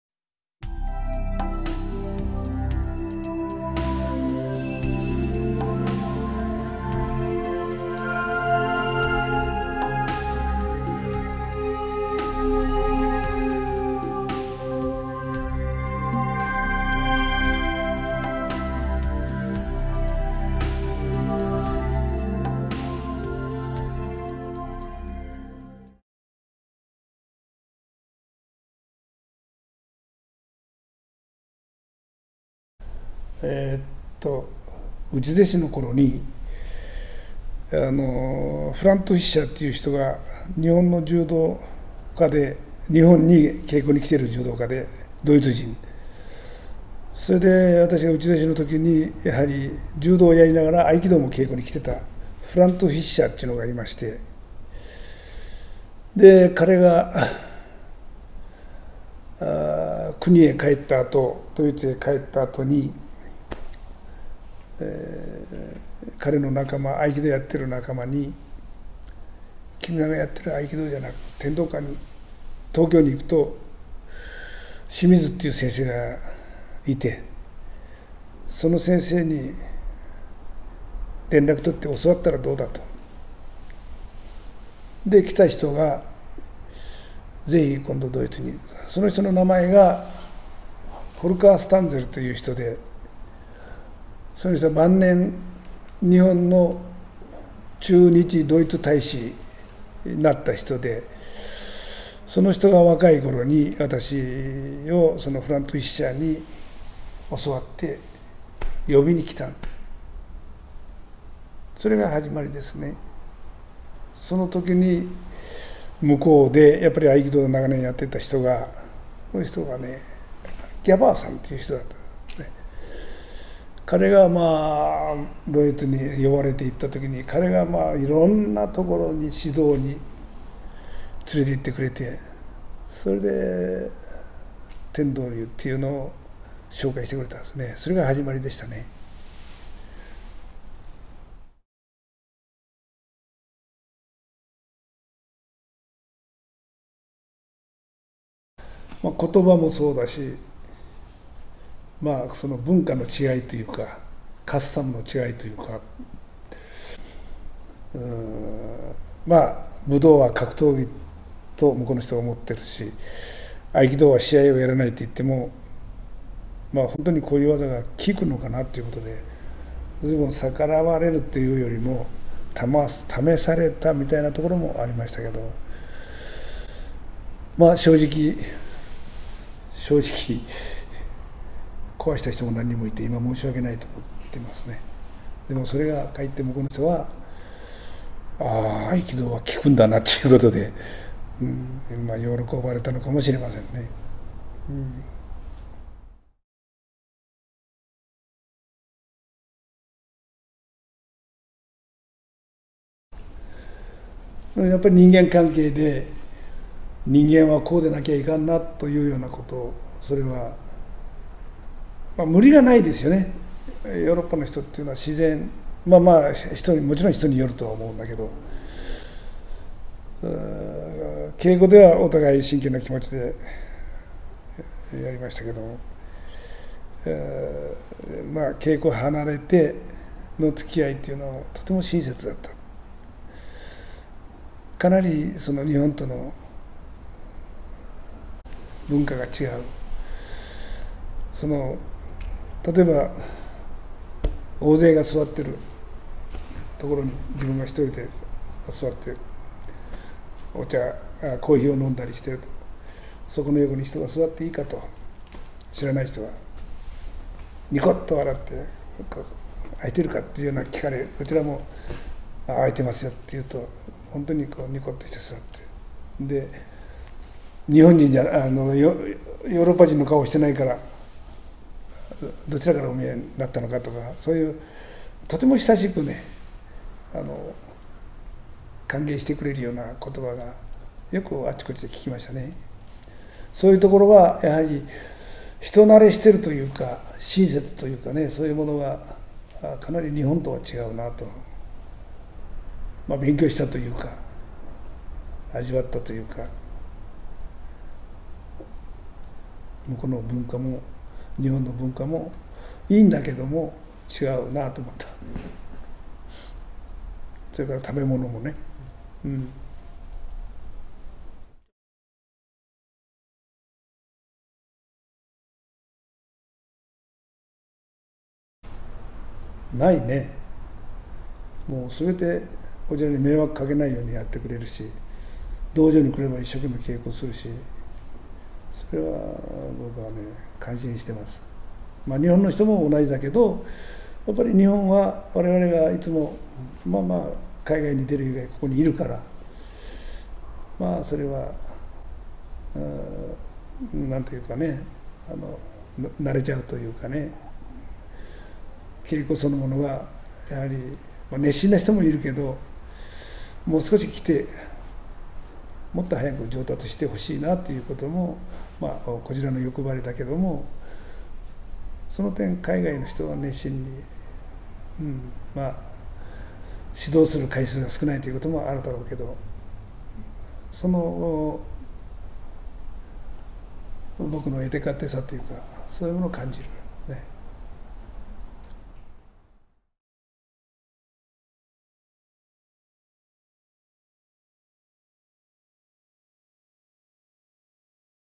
【English subtitles】海外指導の苦労話・海外門弟への想い... 合気道天道館 / 清水健二管長Q&Aインタビュー Vol.10
★0:27 最初に海外指導へと行かれた経緯を教えて下さい -Can you tell us particulars of how it came to your first seminar abroad?
★2:23 海外指導の中で一番苦労したことは？-What was the most troublesome part in teaching abroad?
★3:19 海外指導を経験され学んだことはありますか？-Have you gained some special experience by teaching abroad?
★5:13 今後海外の門弟に望むことを教えて下さい -Is there any request you have to the students from abroad?

天道流合気道の国内外の門人さん達よりご質問を頂戴し、天道館管長・清水健二、道場長・清水健太がお答えしていきます。
清水健二管長によるQ&AインタビューVol.10です。
管長の海外指導の経緯、海外の門弟への想いについて語って頂きました。
尚、あくまでも天道流合気道門人から質問を受付けており、同門人のために制作した動画であるということをご了承下さい。

合気道天道館
〒154−0004 東京都世田谷区太子堂2−13−15−2階